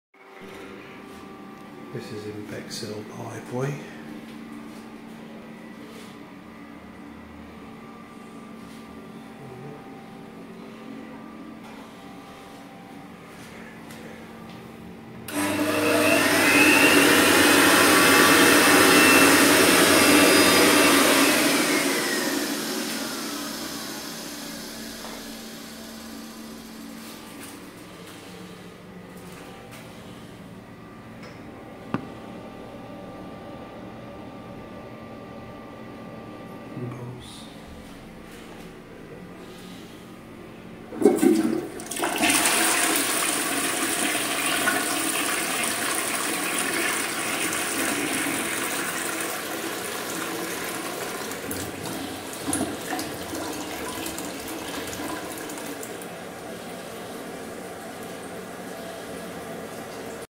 Toilet at the Library in Bexhill
The Library undertaking building works and the Toilet was out of order for a few weeks into a month and I had feelings they did up the Toilet but They didn't it's still the same so I thought I'd film it as it maybe my last time seeing it as it is this is the only public toilet in the Library and it's on Ground floor it would be great if there's another Toilet upstairs for the public then it will end the queuing